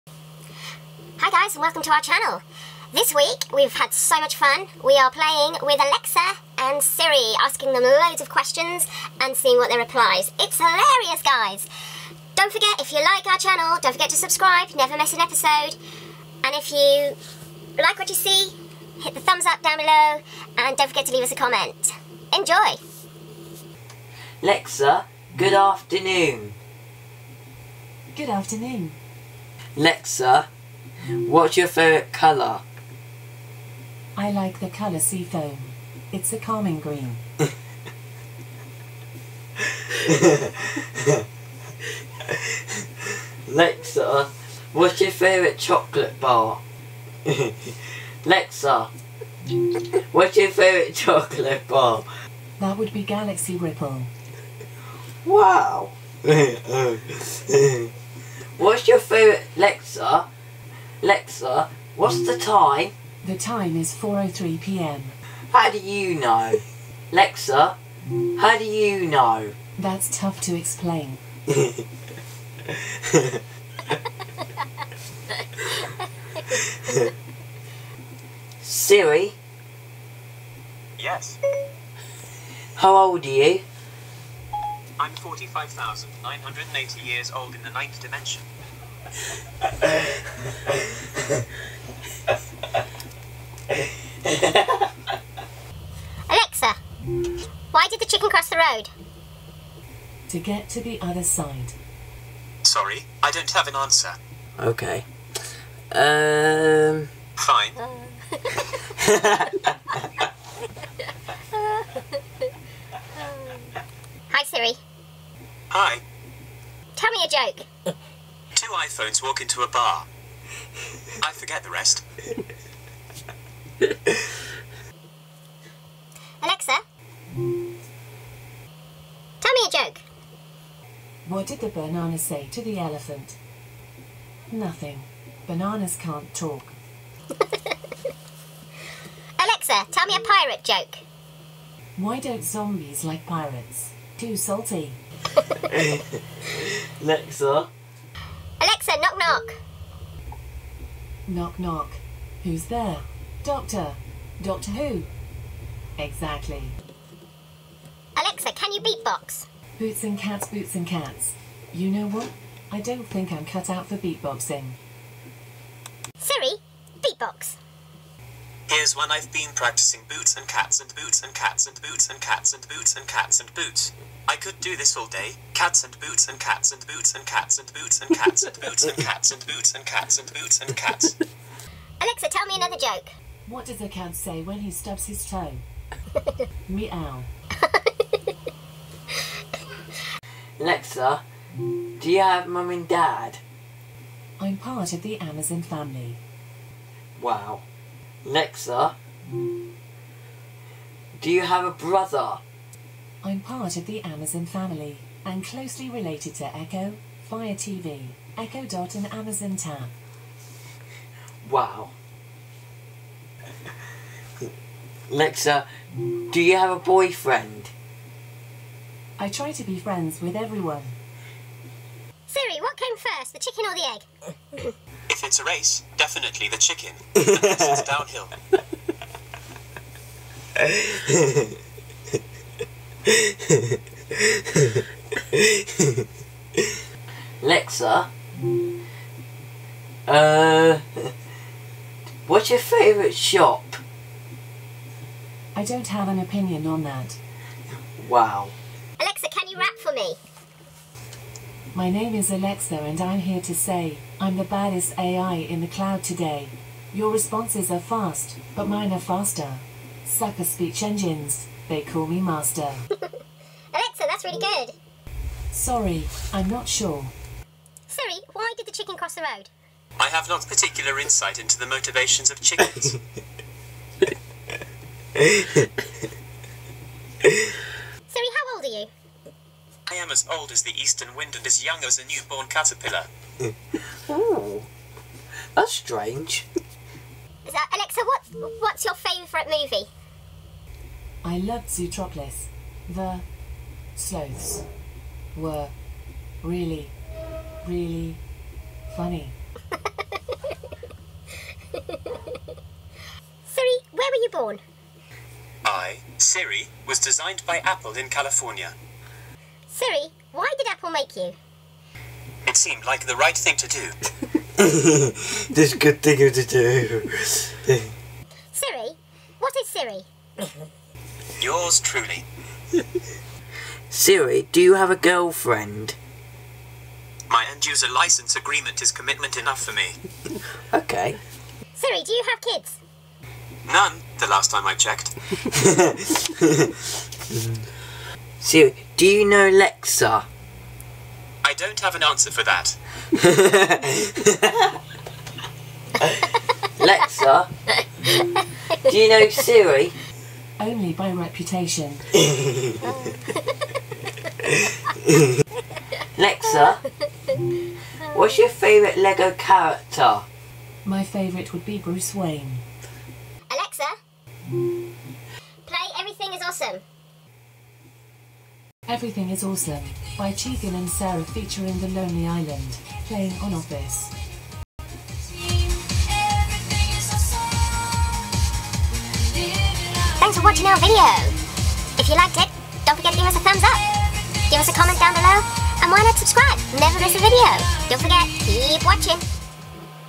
Gigi and I have a laugh chatting with Alexa & Siri
Mr L AKA Legoman and Gigi have a laugh questioning Alexa and Siri. They are in stitches most of the time, what will their answers be. !!

mrlsadventures

FOLLOW US @mrlsadventures